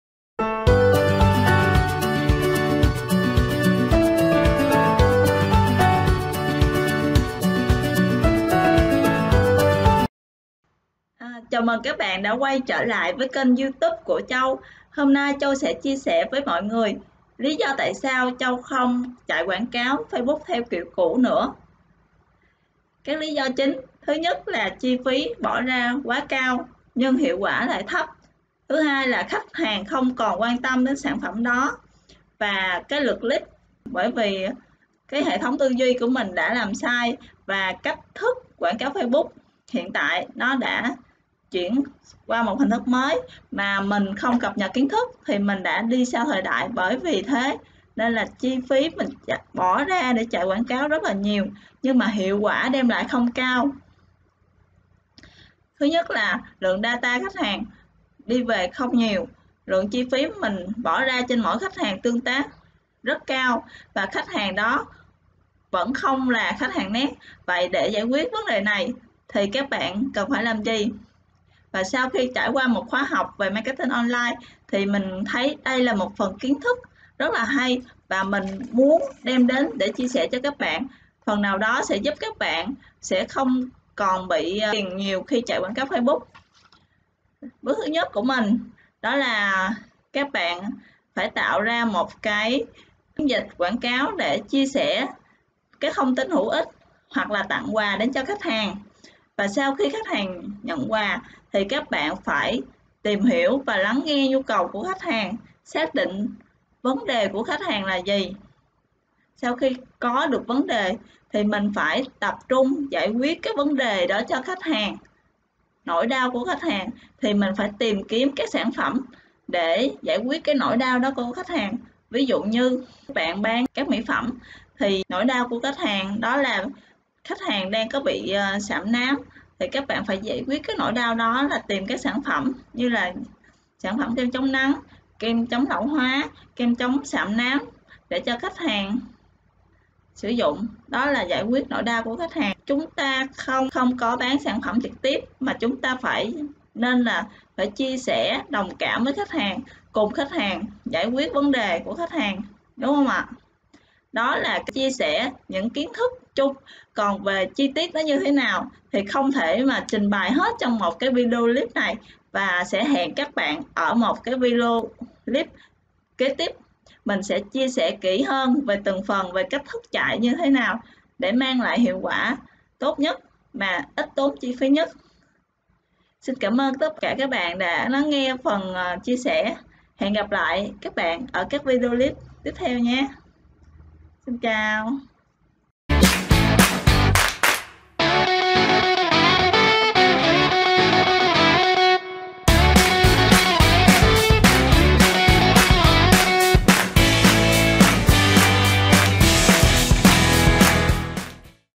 TẠI SAO TÔI KHÔNG CHẠY QUẢNG CÁO FACEBOOK THEO KIỂU CŨ NỮA
Bạn băn khoăn tại sao mình chạy quảng cáo nhiều mà khách không hồi âm,  chỉ click xem chứ ko mua hàng
Hãy liên tục theo dõi các videoclip của Châu nhé
Kết nối với Châu qua Facebook này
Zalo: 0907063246